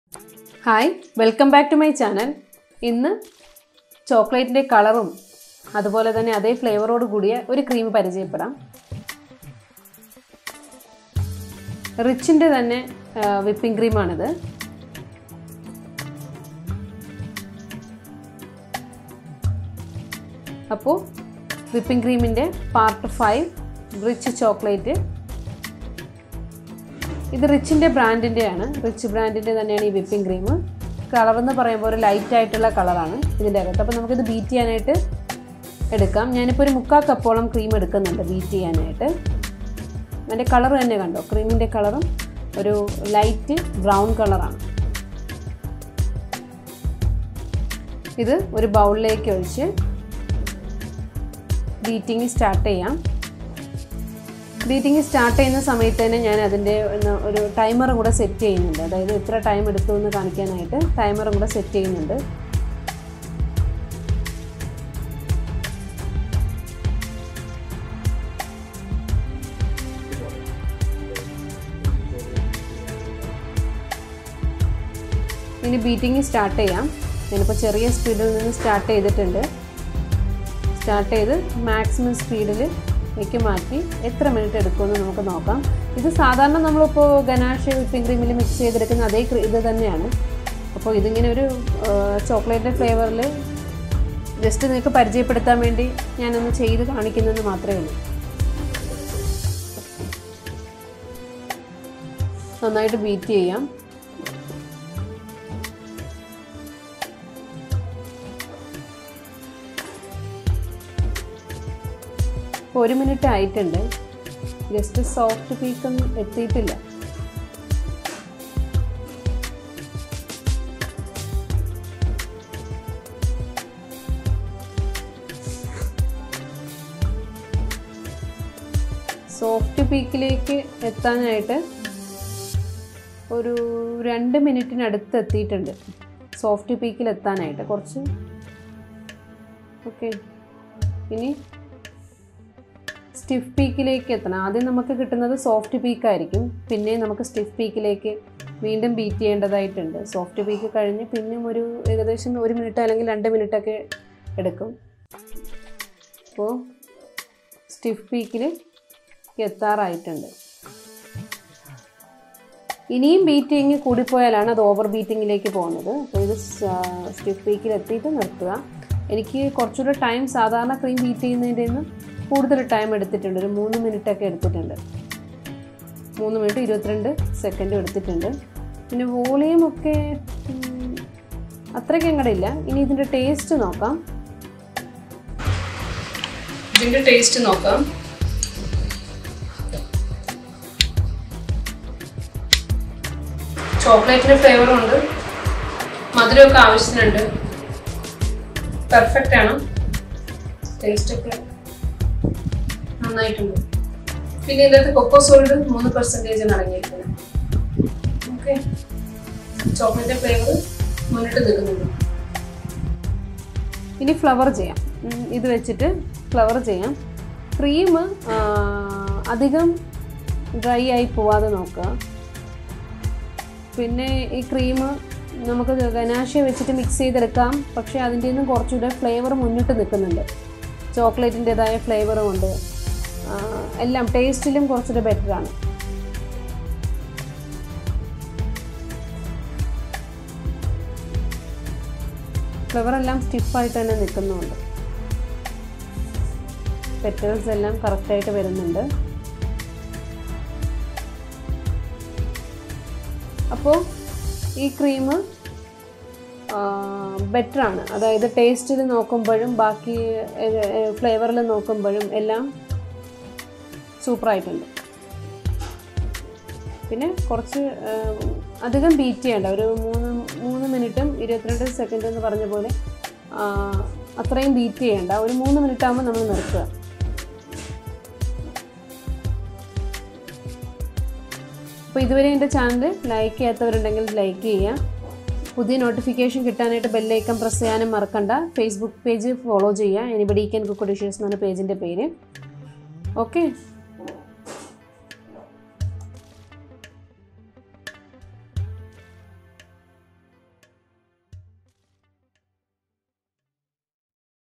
Richs Chocolate Whipping Cream
Richs Chocolate Whipping Cream Review | Best Whipping Cream - Part 5
Which is the best whipping cream brand ?
We will be evaluating and comparing all the creams to find an answer if possible.
Fiona vs Value Star vs Tropolite Vs Pillsbury vs Rich's Daily reviews on each cream
Today we will review  Whipping Cream on the following parameters
1. Time to whip for stiff cream 2. Volume after whipping 3. Taste & Flavour 4. Smoothness & handling cream for decorations Final comparison of all cream will be done and explain the advantages & disadvantages of each cream in the final video of this series.